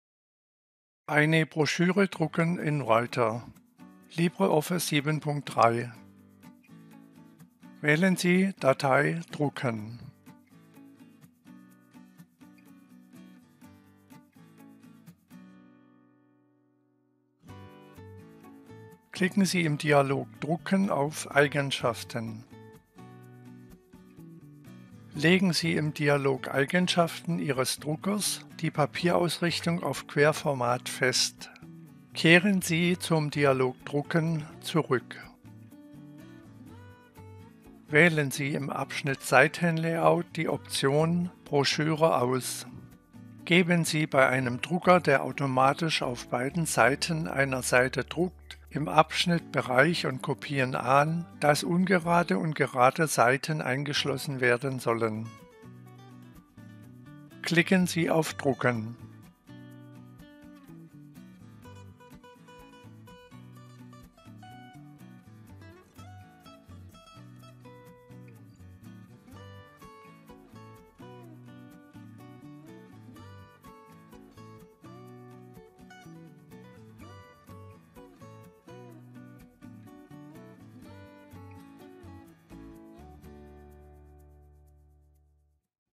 Eine Broschüre drucken in Writer - LibreOffice 7.3 (German/Deutsch)
Dieses Video zeigt, wie eine Broschüre in Writer gedruckt werden kann.